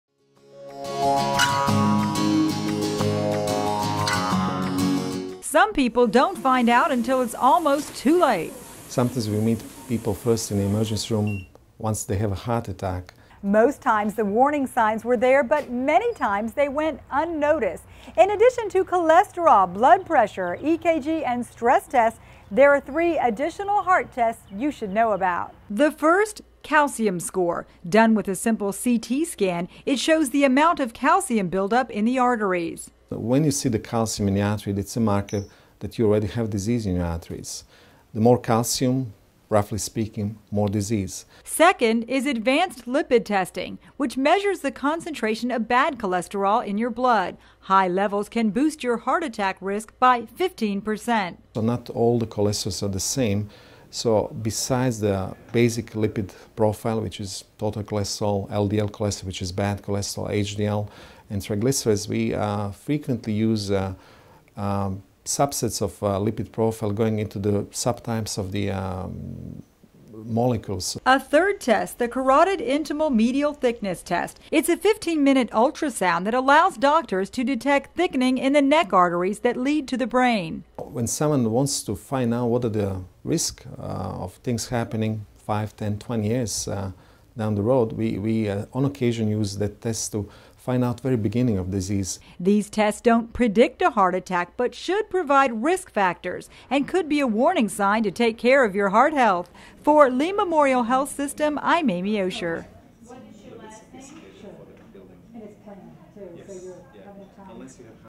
Heart Tests You Should Know About
Heart health: Are you getting all the information you need to determine your risk factor for a heart attack?

Each year, more than a million Americans suffer a heart attack, about half of those occur outside a hospital. That suggests at least some of the victims didn't recognize the warning signs.

In addition to blood pressure, cholesterol, EKG and stress tests- there are 3 additional heart tests you should know about.

First: Calcium Score
Done with a simple CT scan, it shows the amount of calcium build up in the arteries.

"When you see the calcium in the artery it's a marker that you already have disease in your arteries," says Dr. Vladimir Ilic, a cardiologist with Lee Memorial Health System. The more calcium, roughly speaking the more disease."

Second: Advanced Lipid Testing
It measures the concentration of bad cholesterol in your blood. High levels can boost your risk of heart attack by 15%.

Third: Carotid Intimal Medial Thickness Test
An ultrasound that allows doctors to detect thickening in the neck arteries that lead to the brain.

"When someone wants to find out whether their risk of things happening 5, 10, 20 years down the road, we on occasion use this test to find out very beginning of disease," says Dr. Ilic.

These tests don't predict a heart attack, but should provide risk factors and could be a warning sign to take care of your heart health.